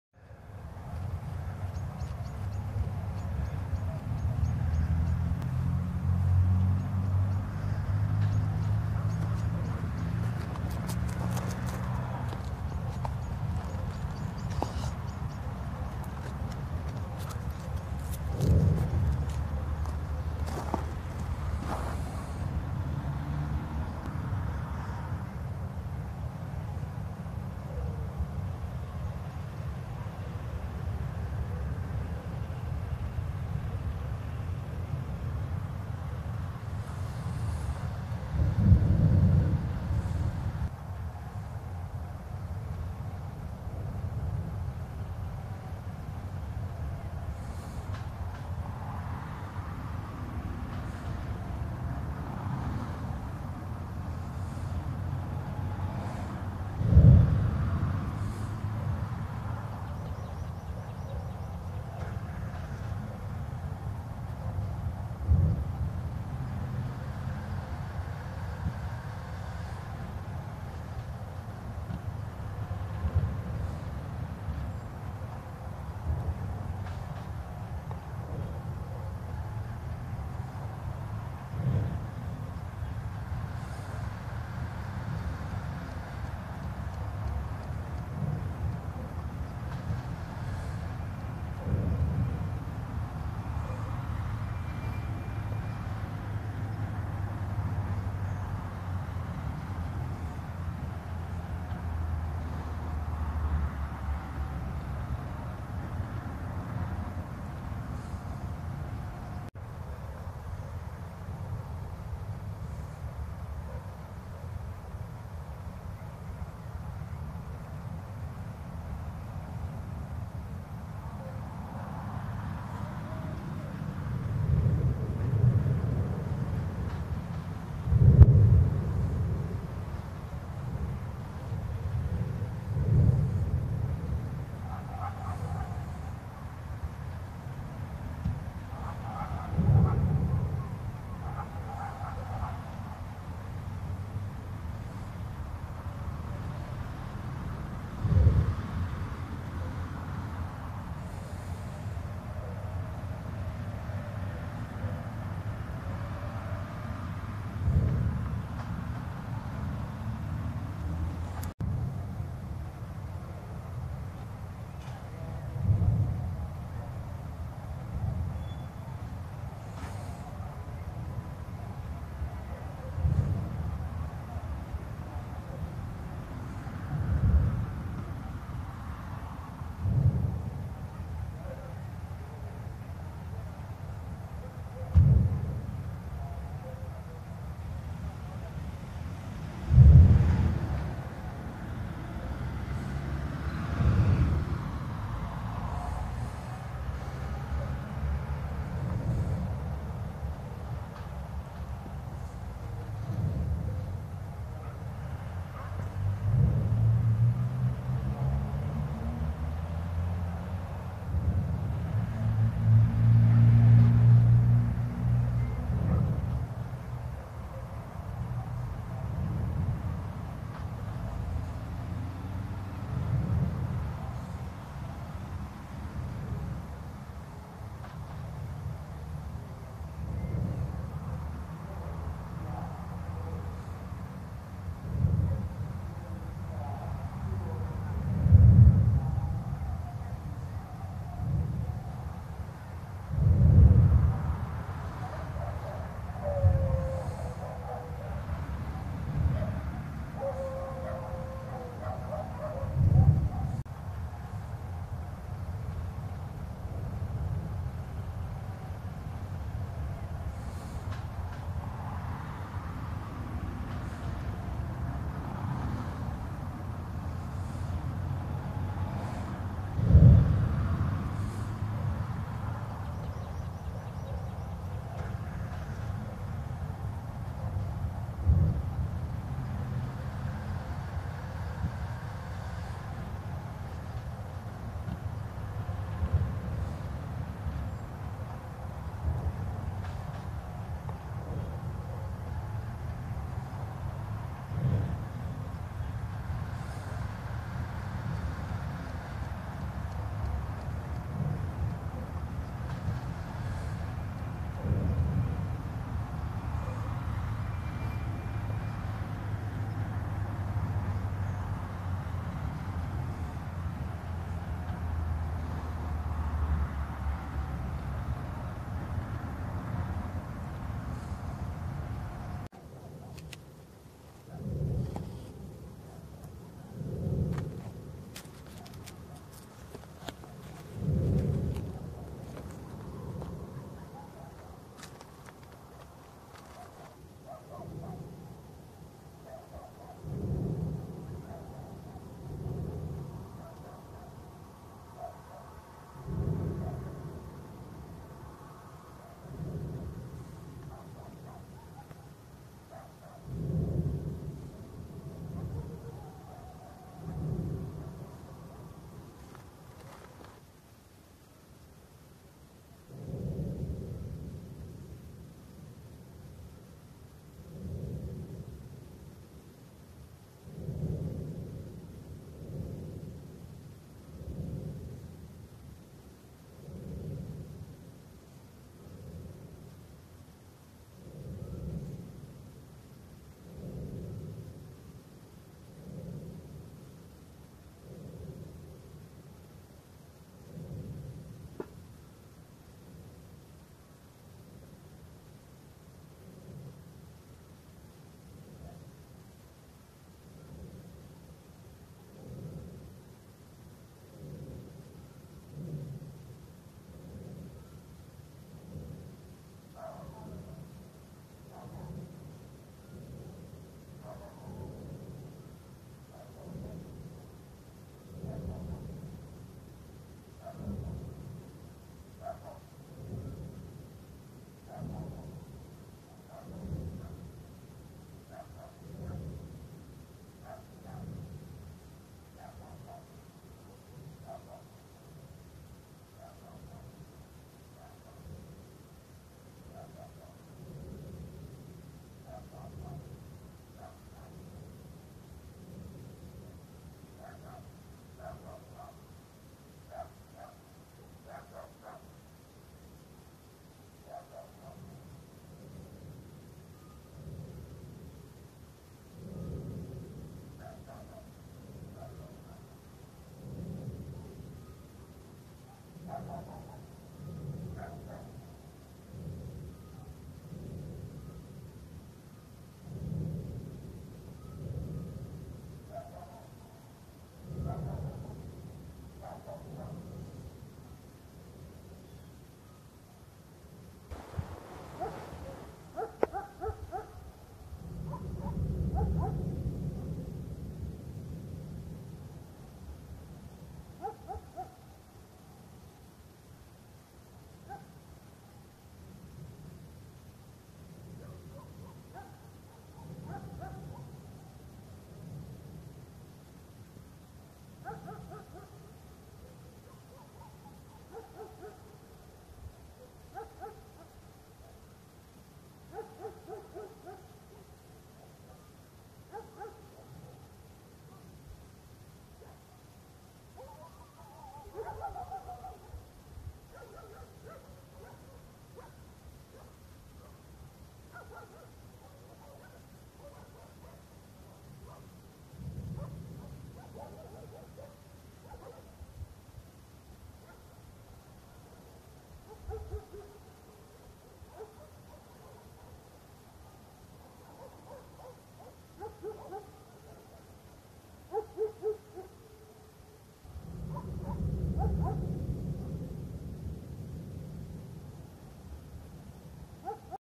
Lava fountain at Etna's New Southeast Crater, 27 April 2013
(This is a re-upload, because I did an editing mistake on the original video post - 31 March 2015.) Video recorded from Santa Venerina, Pisano, Fleri and Trecastagni, between 18:00 and 19:45 GMT (local time -2) on 27 April 2013.